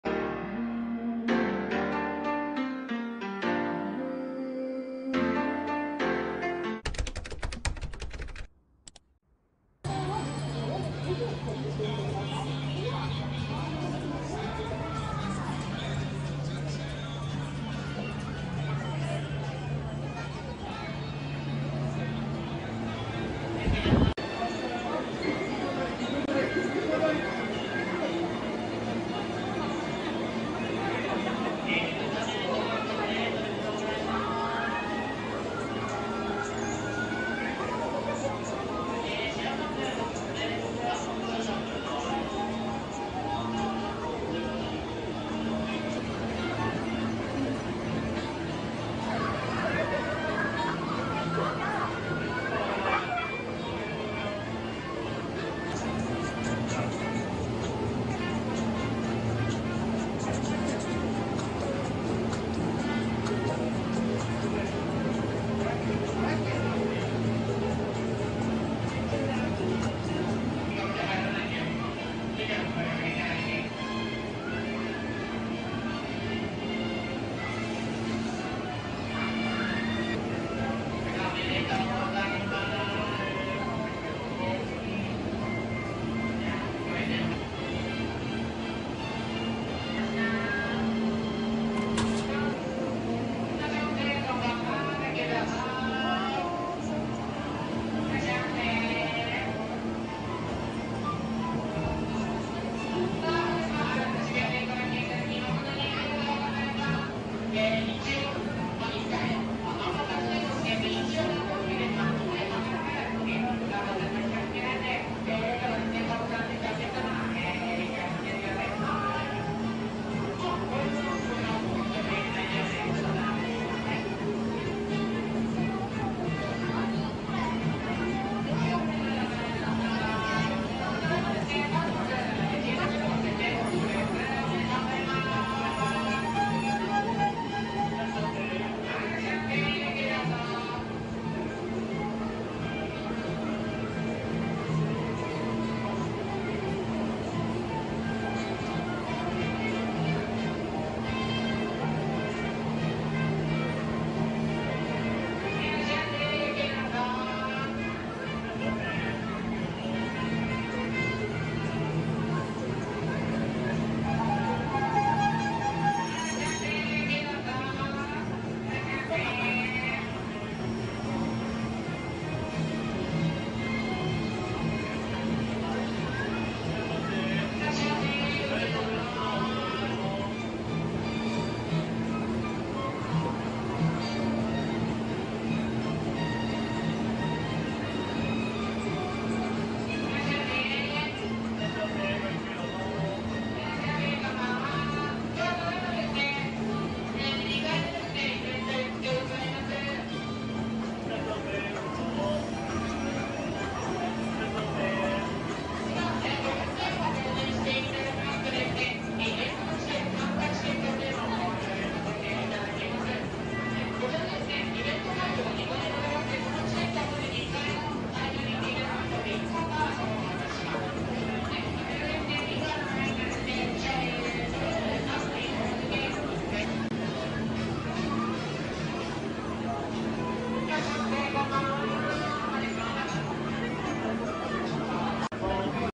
Cotton Candy Machines are very popular in Japan. The profits are high.Customer feedback video
The profits are high.Customer feedback video